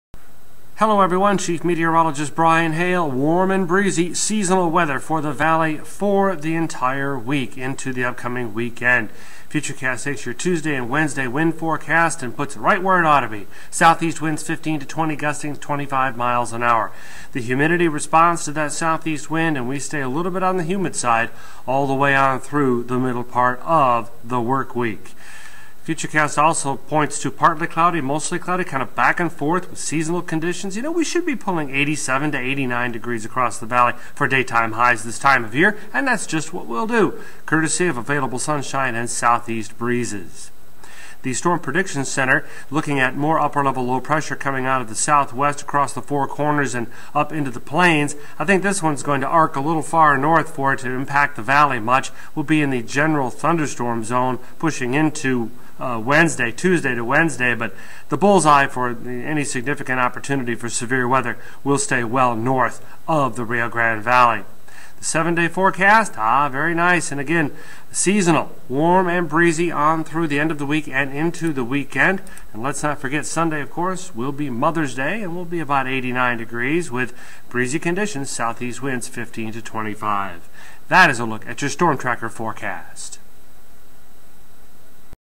Bryan Hale's Weather Forecast for the RGV
Seasonal weather across the RGV with plenty of wind and warm weather.